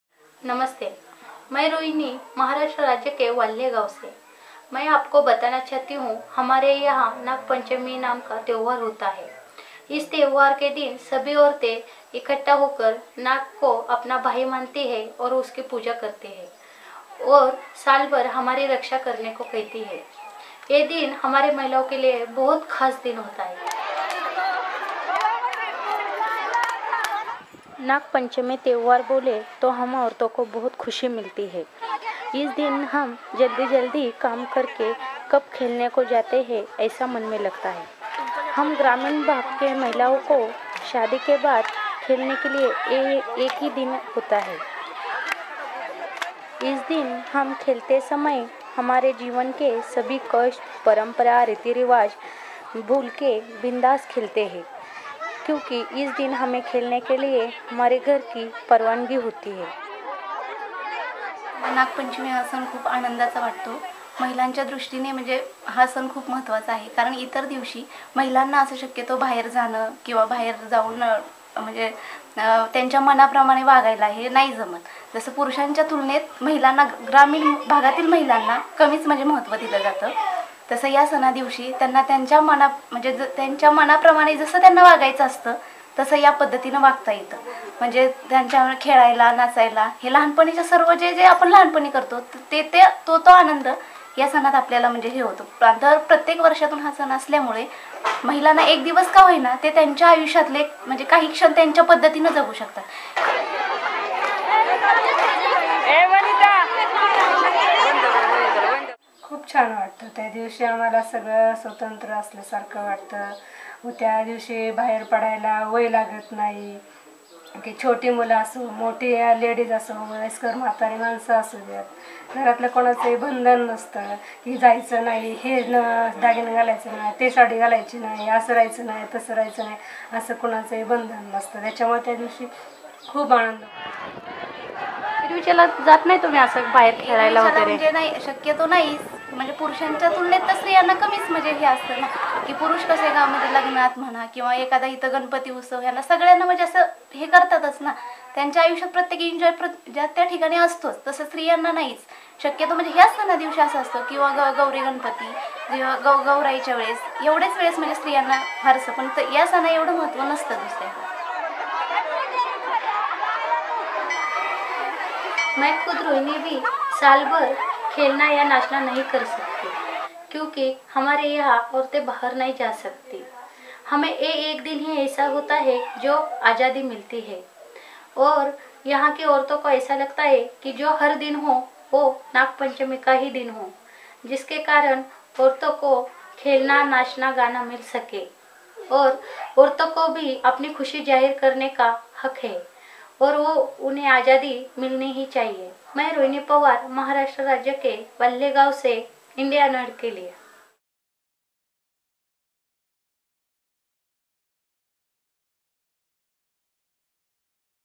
Women Celebrate Freedom Fest
Married women, including the correspondent of this video in India's Maharashtra to play and dance - otherwise considered a taboo - on Nag Panchami. Nagpanchami or the Snake festival has a totally different meaning for women in Walhe village of India's Maharashtra state as the day allows them to dance and play on the occasion of the festival. Rohini gives us an exclusive insight into this tradition that is the only occasion to feel free. In the rural belt of India - a patriarchal society -women labour harder than men do, their areas of work stretching from home and kitchen to the fields. Yet their lives are dogged with problems such as female infanticide/foeticide, early marriage, dowry, forced divorce, malnutrition, poor health and lack of education. These are endemic problems existing in all states, irrespective of religion and communities. In rural Mahasrashtra where Rohini, the correspondent of this video lives, women are treated as inferior to men. So while men are free to do whatever they want, women's moves are restricted and they must take need special permission from family members to even move out of their homes. They are especially forbidden to sing, dance or play in public view. Those who do so, are looked down upon as women of loose morality. However, once in a year -- on the day of Nagpanchami this bar is lifted and women are expected to dance and play games. So this day local women gather at the snake goddess temple to worship. However, what they really wait for is the moment when the worshipping is over and dance and games can begin. Once that moment comes, everyone joins in dancing and playing -- acts that are otherwise considered a taboo. Rohini says that throughout the year women of her community look forward to this day when nobody will shout at them for dancing or, nobody will accuse them of breaking a tradition by playing. However, after this day, they will have to return to the life of restriction again. Rohini feels that this must change. She wants the patriarchal society to change its thoughts and values, so that women like her will not have to enjoy for one long year to enjoy a day's freedom.